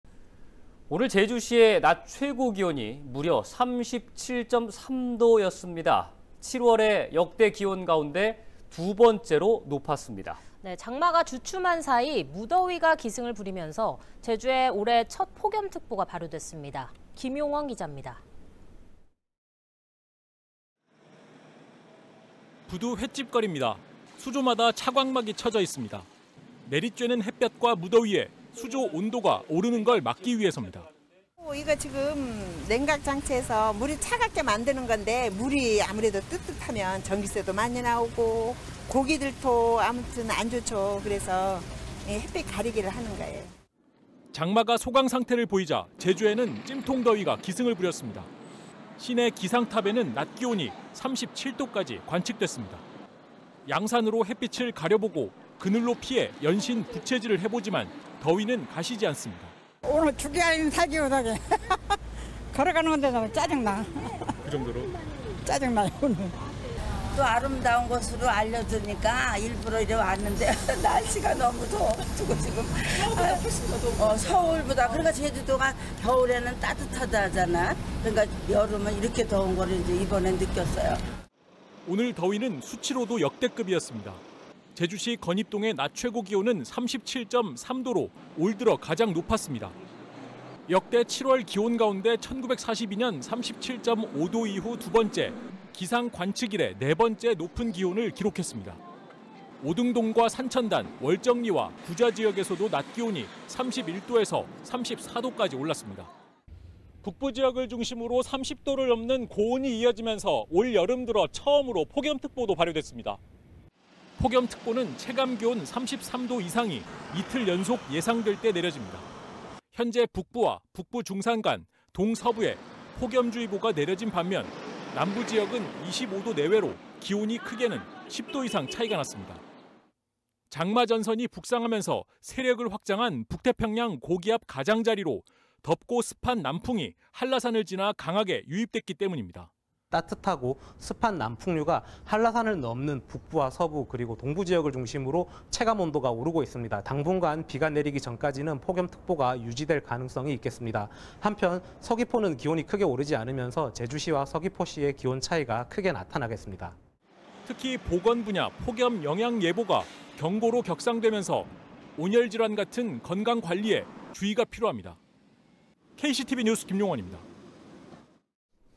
'제주시 37.3도' 7월 역대 2위 …첫 폭염특보
오늘 제주시의 낮 최고기온이 무려 37.7도를 기록했습니다.

7월 역대 기온 가운데 두번째로 높은 것입니다.

장마가 주춤한 사이 무더위가 기승을 부리면서 제주에서 올해 첫 폭염특보가 발효됐습니다.